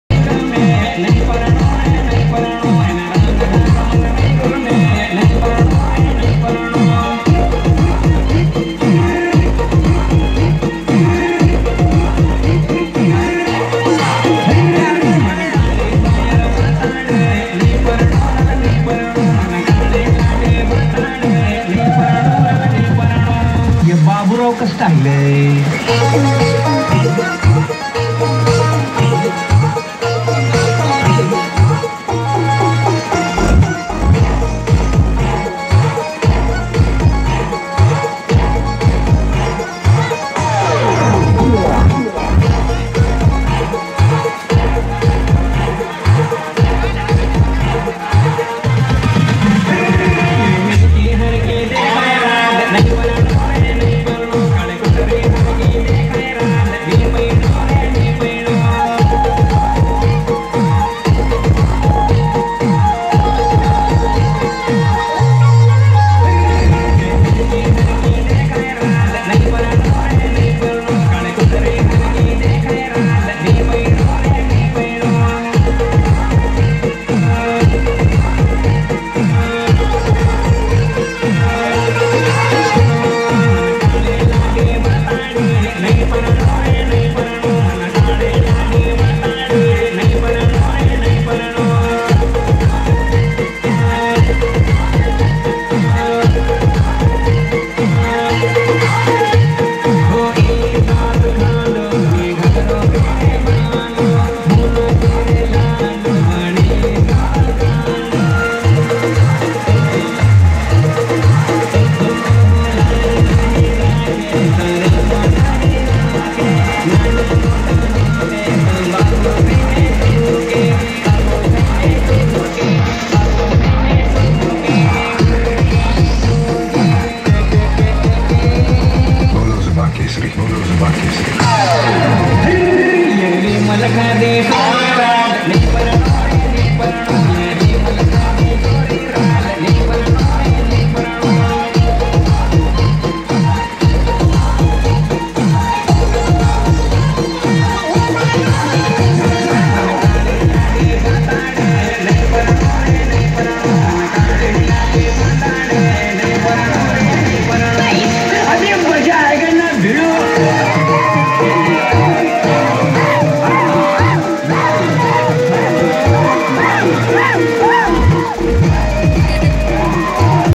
New Ansing Katija Timli Song Dance // 2022 // Ansing Katija
AK  STATUS KING

                     New Video

Singer :-ANSING KATIJA & Rahul Bhuriya

Cameraman :- AJAY AMALIYAR

EDITING :- AJAY AMALIYAR

Dancer boy :- Pravin Damon , Rahul Damor,  Pankesh Sangada,  Mahesh Amaliyar,

           AK STATUS KING 👑

Next Video Status